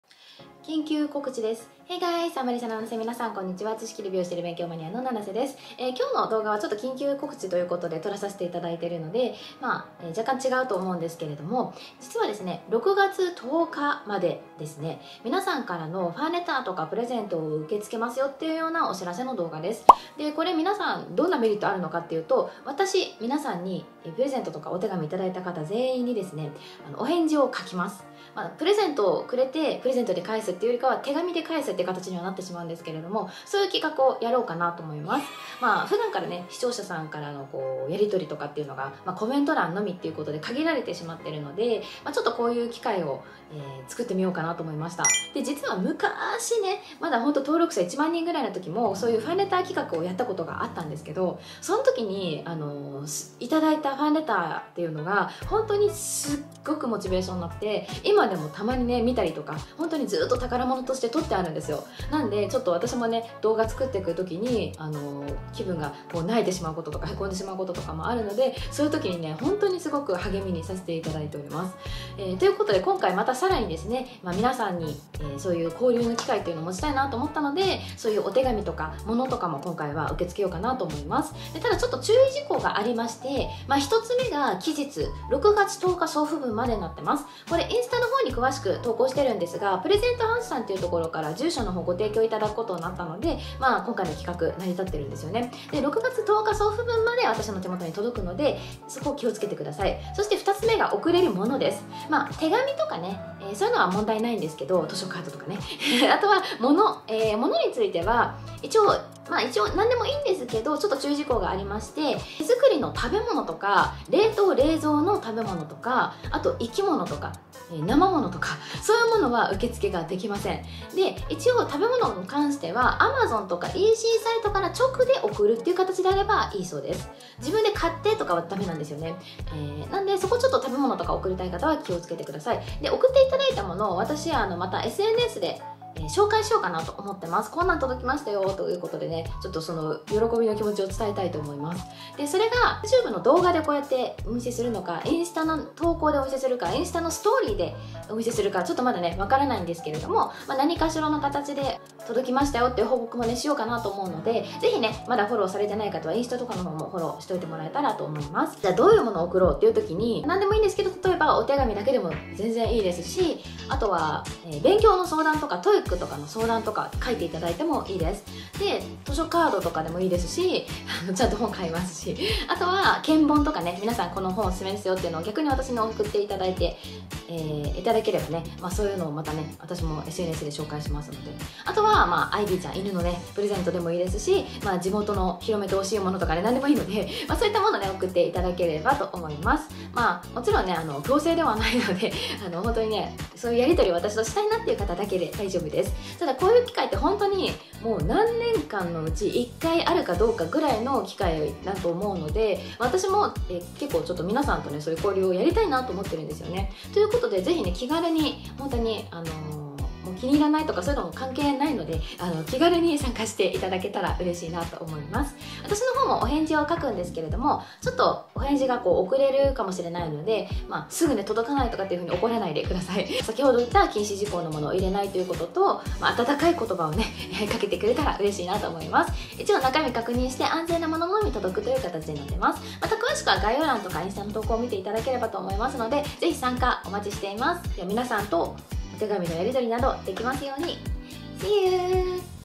緊急告知【6月10日まで】＊数年に１度の交流企画❤️
今までも過去に１度だけ、今回もこの機会を逃すと次はいつこういう企画をやるかはわかりません。
2023年6月10日までとなりますが、
ぜひ参加してくれると嬉しいです！（一通もこなかったらどーしよ😭笑）

※いくつかルールをよく読んでね▽​
【期間】6月10日送付分まで​
【送れるもの】手紙、物など​
(例えば、ちょっとした悩み相談やTOEICの勉強相談、図書カード、献本、アイビーへのプレゼント、広めてほしい地元のものなど…amazonなどECサイトから直でもok!)​
【禁止事項】手作りや冷凍冷蔵の食べ物、生物は受け付けられません。中身を確認し安全なもののみ手元に届きます。​
【お返事】いただいたものに、ご自身の返信用住所を書いた封筒も入れていただけたら助かります！​
また、順を追ってお返事しますので、1ヶ月程度かかる場合がございます。​
プレゼントの場合も、お手紙でのお返事となります。​
【留意点】個人が特定できない範囲でSNSでも紹介させていただく場合がございます。
ちゃんと七瀬の元に届いたかご確認ください。(instagramのストーリーor投稿or youtubeのいずれかに投稿します)​
むか〜し一度だけファンレター募集企画をしましたが、今でも送っていただいたお手紙は宝物です。​
私のモチベーションになってます😍​
今回は物もokなので、(送料がなるべくかからないもの)いただいたものは大切に使わせていただきます！✨​
あたたかいお言葉お待ちしてます😭❤️​
送り先住所は下記となります▽​
〒573-0073​
大阪府枚方市高田2丁目26-10-6​
プレゼントハウス350「七瀬アリーサ」​

七瀬アリーサの記入を忘れずに！